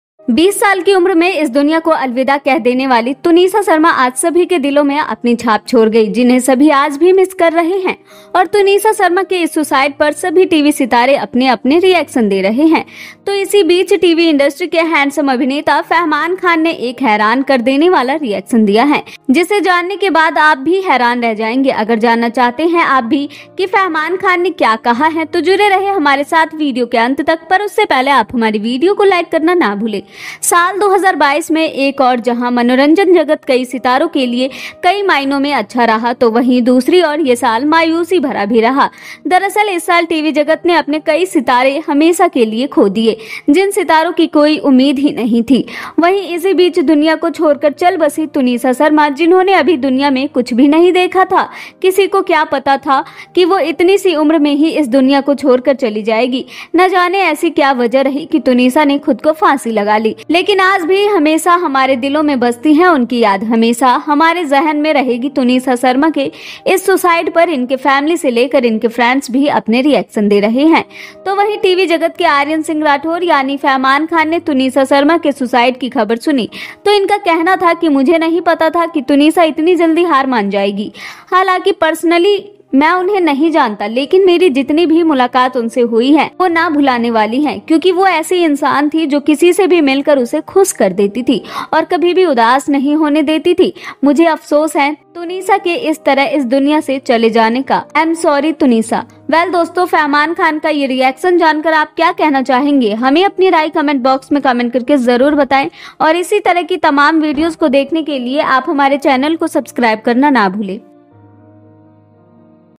तुनीशा शर्मा के खुदकुशी करने के बाद फहमान ने कहीं चौंकाने वाली बात। Tanisha go away in fahman tolk..
After the suicide of Tunisha Sharma, Fahmaan said something shocking. Tanisha go away in fahman talk